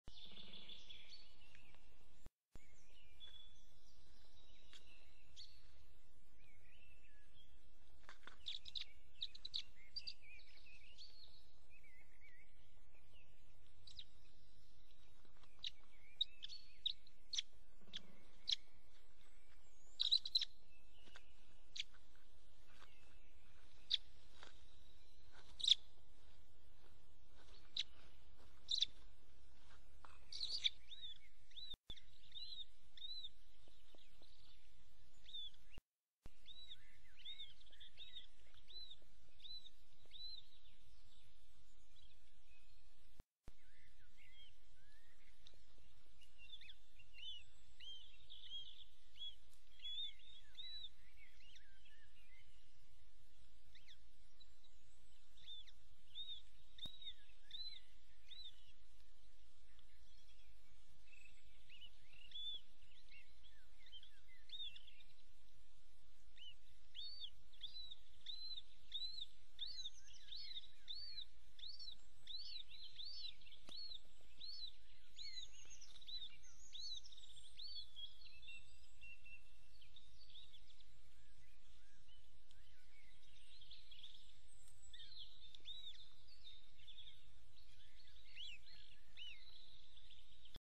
Pisklę orła 🐣 Estonia - 25 04 2022
Pisklę już było karmione.
Jajka zniesione były 15 i 19 marca.